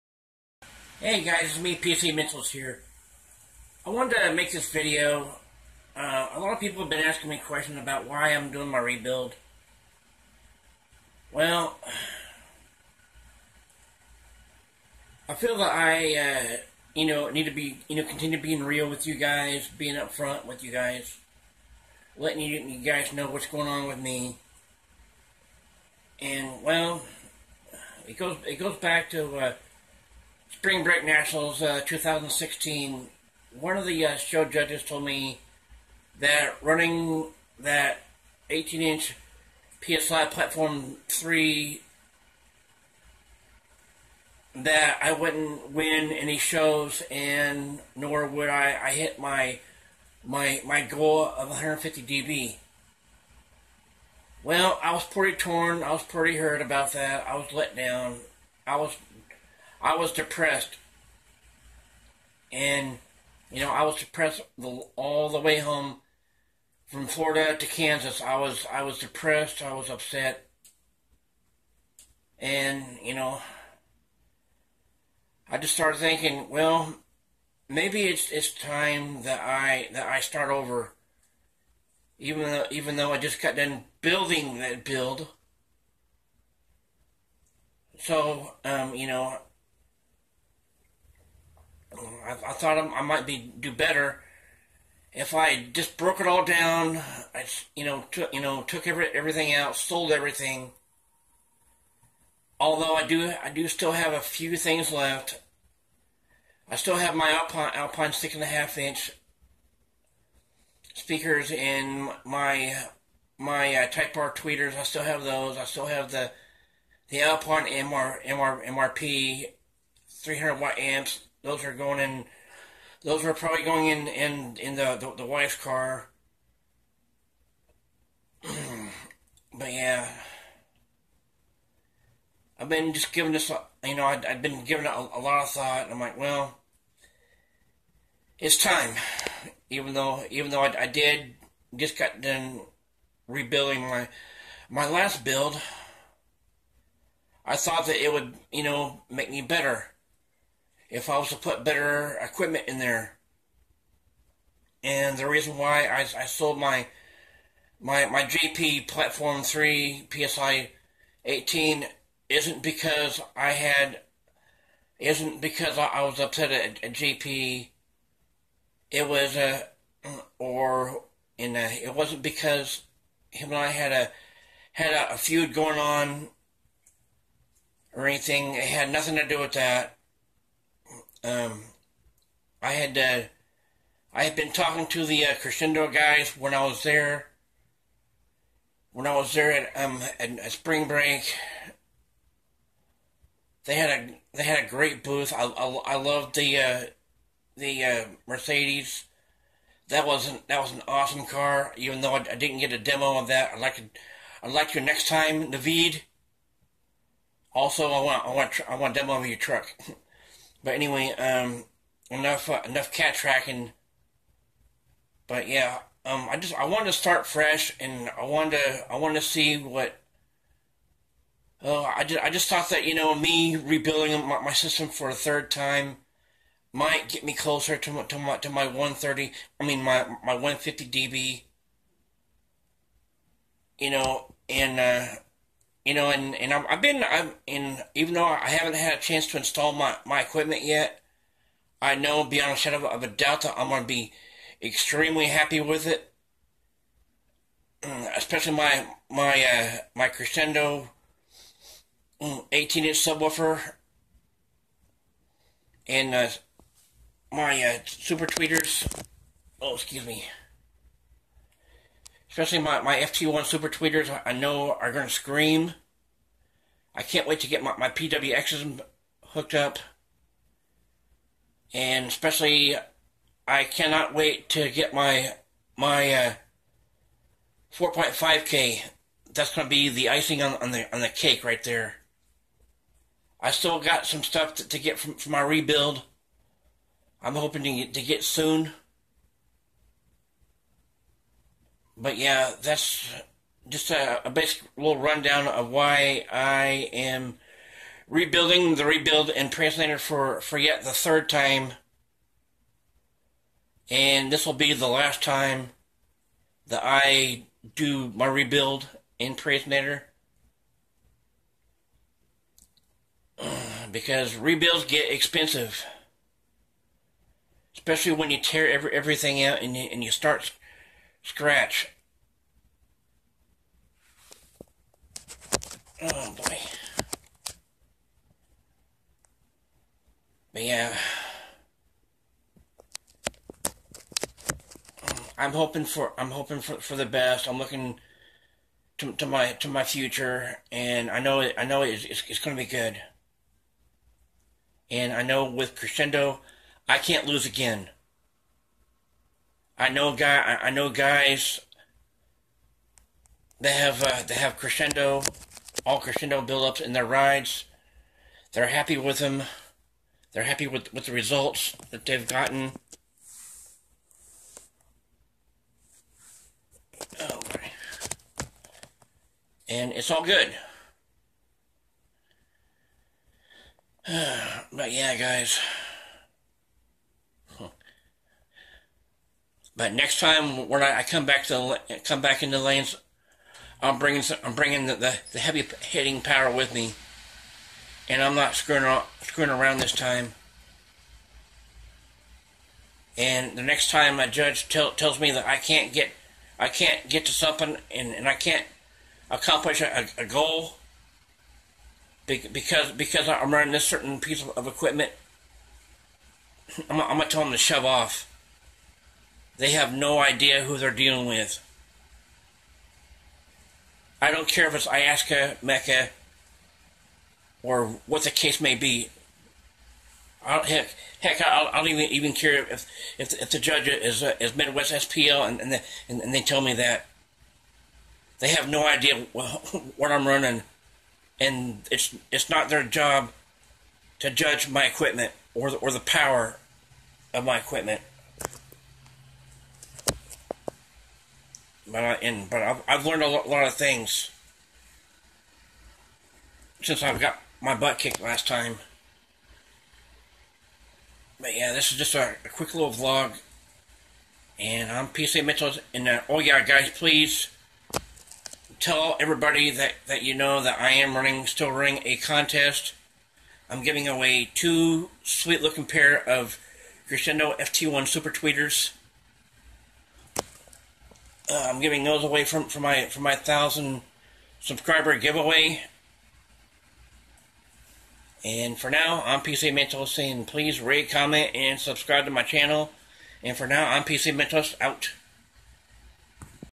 Basshead answer to why I'm doing my rebuildup of my system
Just a little video vlog of why I'm doing my rebuildup of my system answering a question that's been asking over and over again.